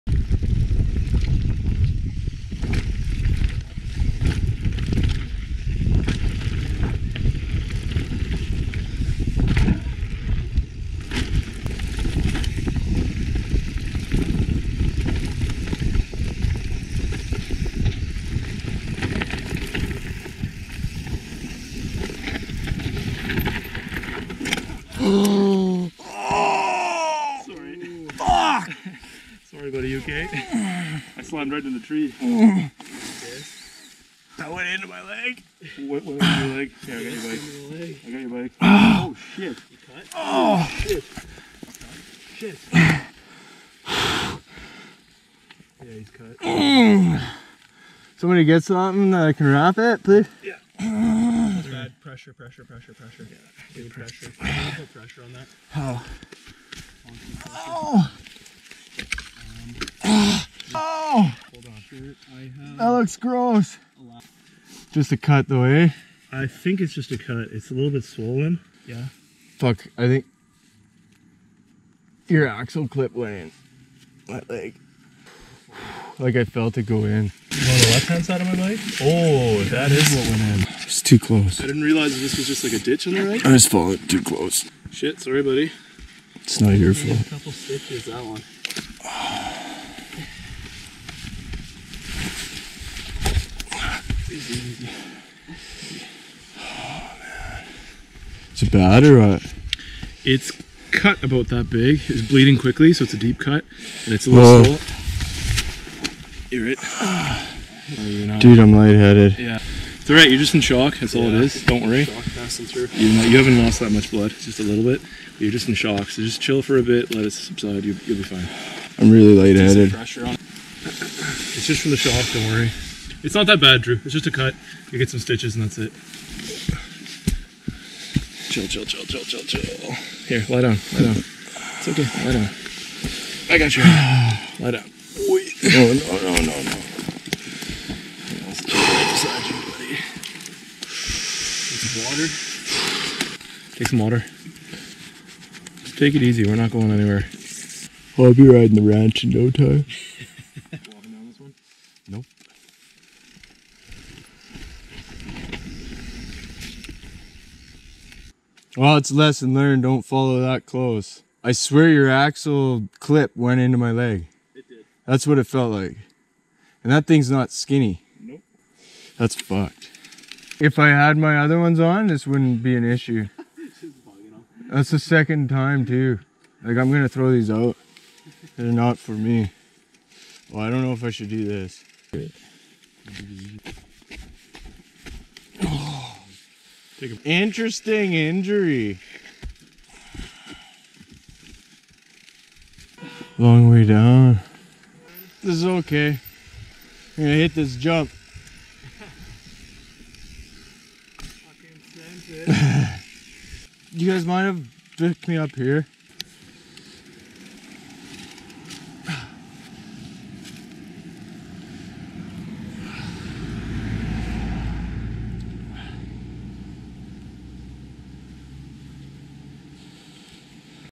I'll Never Follow That Close Again! // A Painful Day at Harper Mountain
Lessons learned after taking buddy's axle clamp to the shin:
- Don't follow stupid close, you never know what can happen!
- Second time this month these shins failed me, ordered new ones.
- Pack a first aid kit.

This was our last lap at Harper before heading to the Bike Ranch. I was wearing my TLD slip on knee/shins and they weren't quite long enough to stop it.